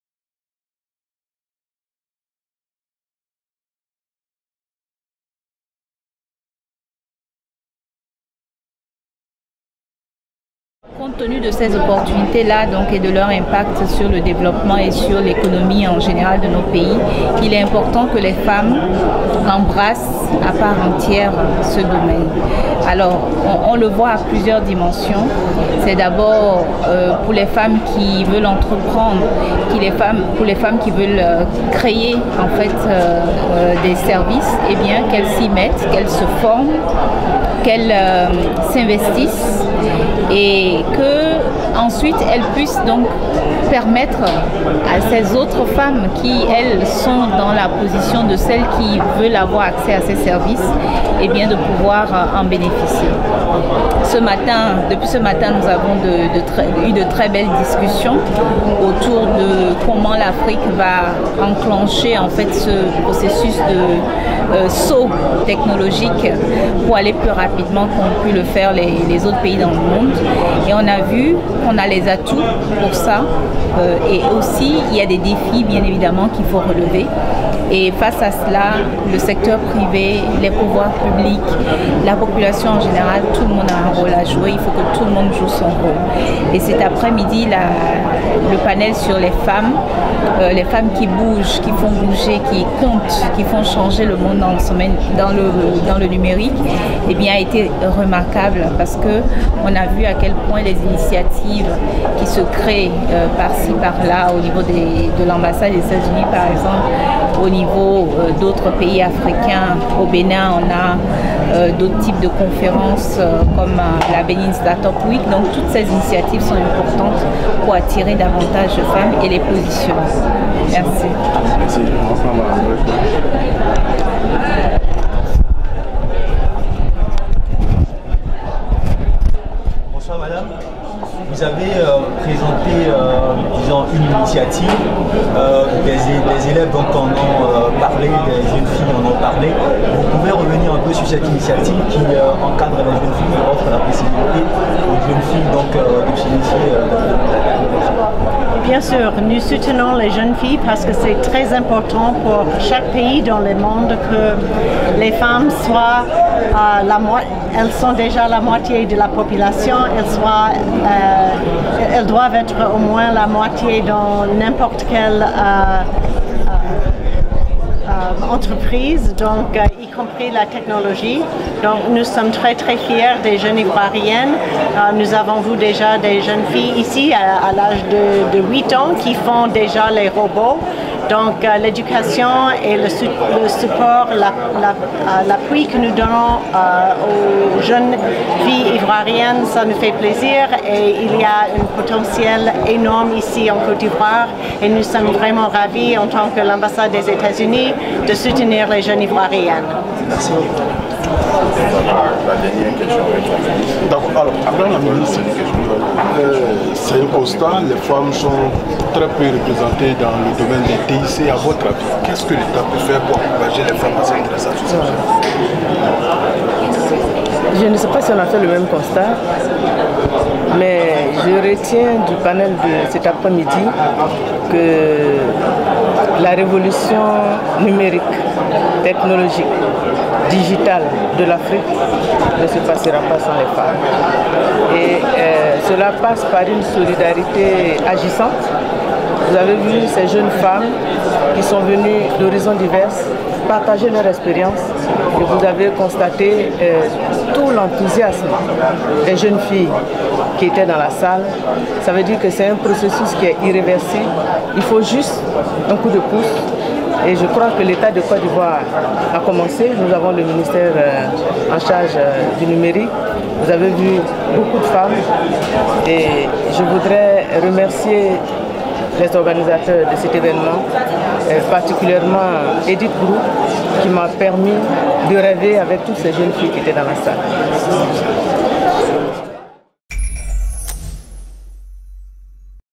interview femme d'honneur African Women 4 Tech
akody fait une interview aux femmes leader de African Women 4 Tech